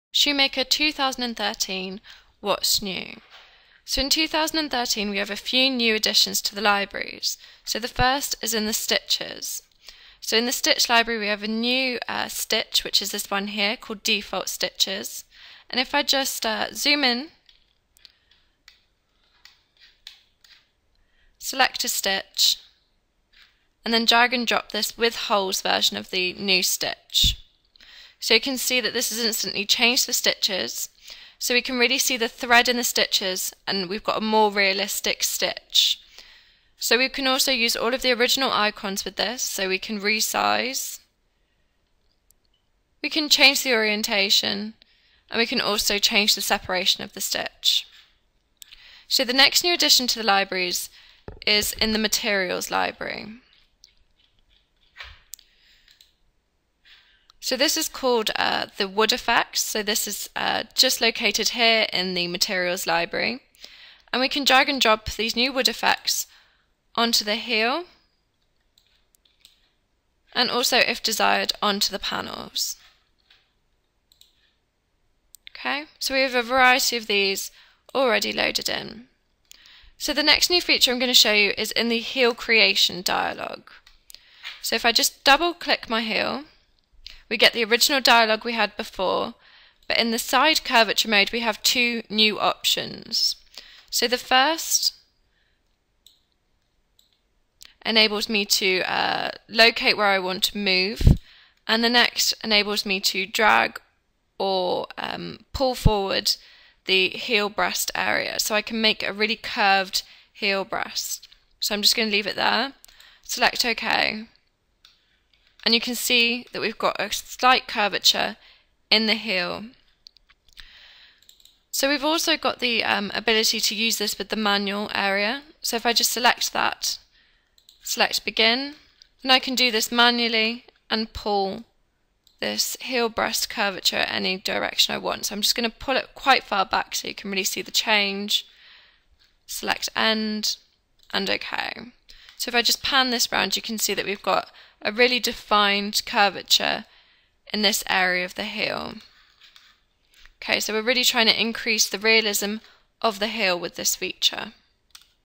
ShoeMaker 2013 R1 - Library Additions and Heel Creation
See new stitches library, materials library and heel creation functionality in 3D shoe design software, Delcam CRISPIN ShoeMaker.

Delcam CRISPIN ShoeMaker provides sophisticated yet intuitive design tools so you can produce realistic 3D digital models. This allows you to approve designs in less time and reduces the need for expensive physical prototypes.